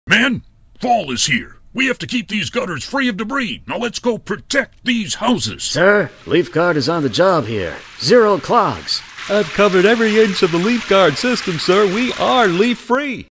LeafGuard- Attack Of The Leaves
Defend your home from the attack of the leaves this fall! Put an end to clogged gutters waging war on your home!